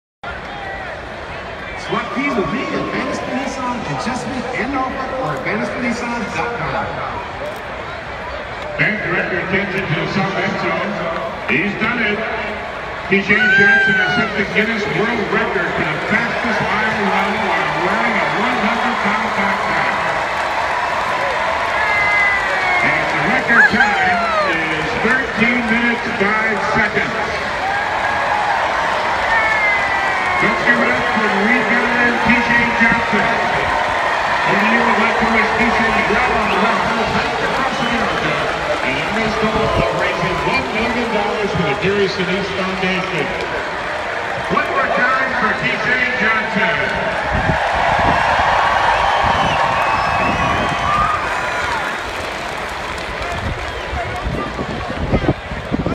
T SHANE JOHNSON
T Shane Johnson breaks the world record for the fastest one mile with a 100 lb pack!  Way to go!